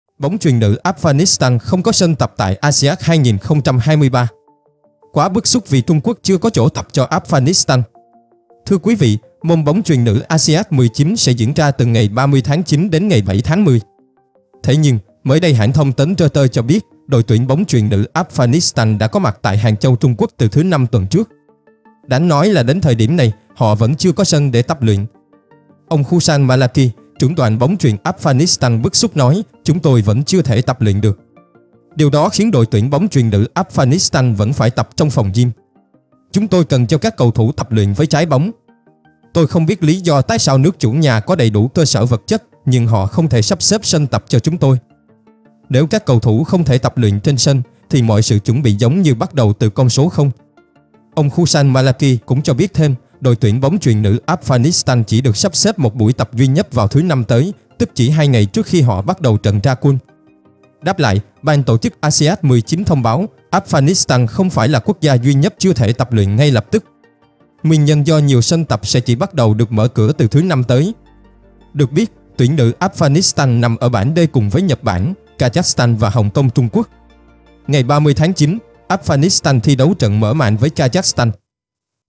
BÓNG CHUYỀN NỮ Afghanistan KHÔNG CÓ SÂN TẬP TẠI ASIAD 2023. bức xúc vì TQ chưa có chỗ tập
BÓNG CHUYỀN NỮ Afghanistan KHÔNG CÓ SÂN TẬP TẠI ASIAD 2023
Quá bức xúc vì Trung Quốc chưa có chỗ tập cho Afghanistan